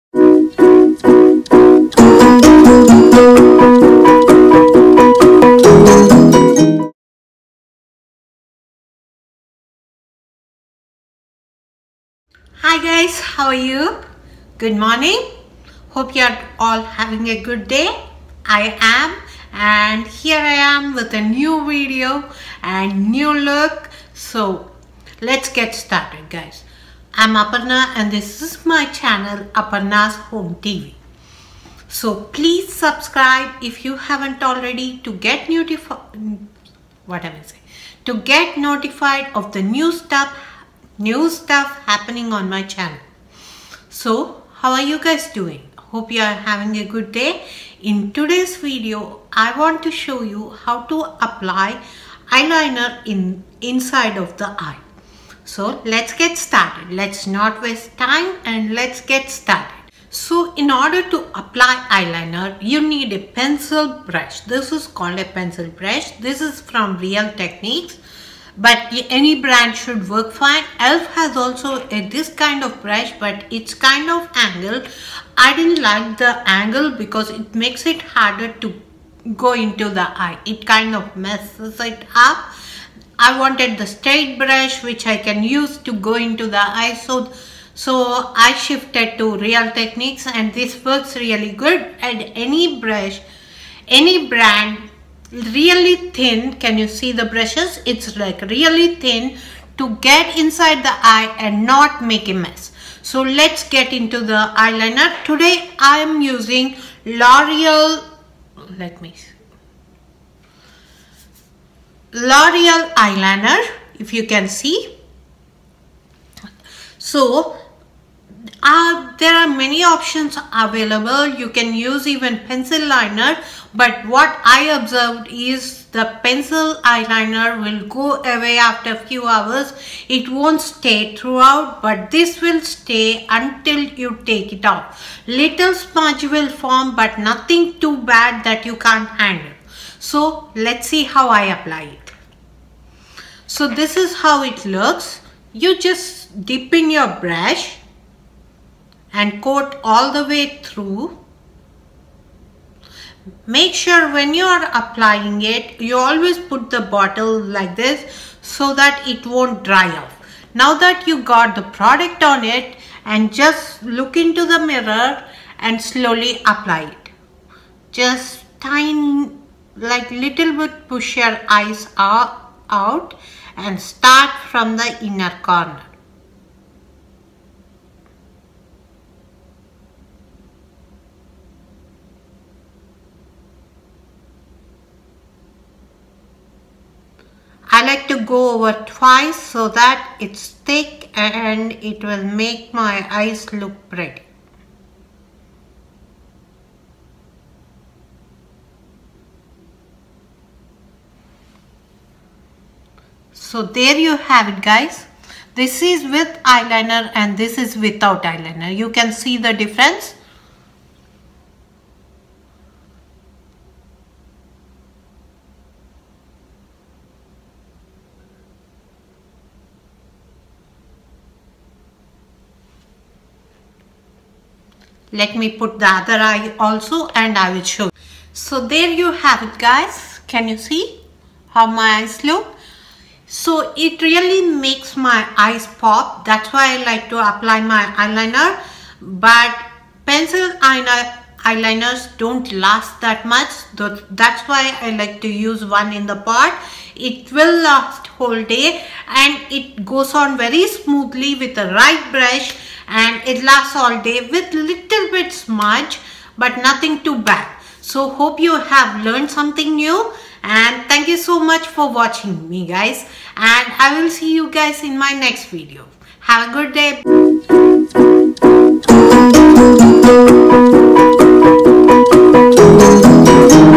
Back to Basics:How to apply Eyeliner in the lower eye? (Quick and Easy Tutorial)
In this video you will learn how to quickly apply eyeliner.

Thanks,
Aparna